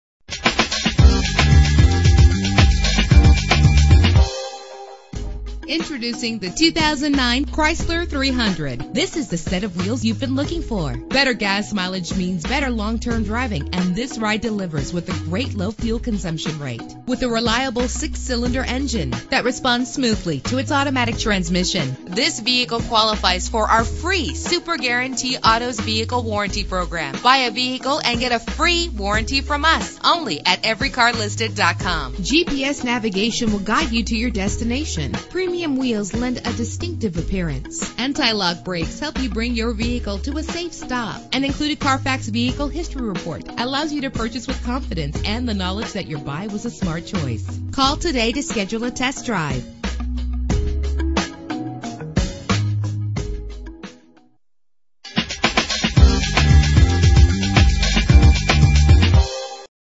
2009 Chrysler 300 - Gainesville GA
Year: 2009
Make: Chrysler
Model: 300
Trim: Touring
Engine: 3.5 liter 6 cylinder 24 valve
Transmission: 4-Speed Automatic
Color: White
Mileage: 33486
Address:
3115 Frontage Rd